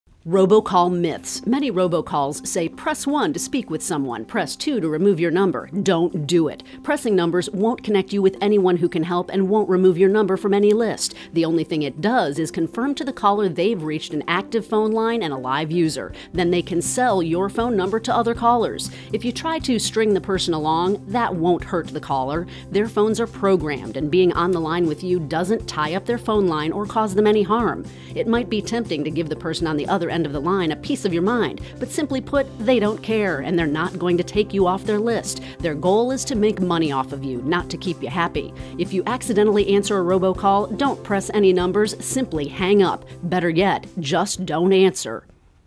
Consumer Protection: Robocall Myths
The Ohio Attorney General's office has created a Robocall Enforcement Unit to make it harder for illegal robocallers to operate in Ohio.

If you receive an unwanted call, text ROBO to 888111. Fill in a few pieces of information and you'll help us find the robocallers.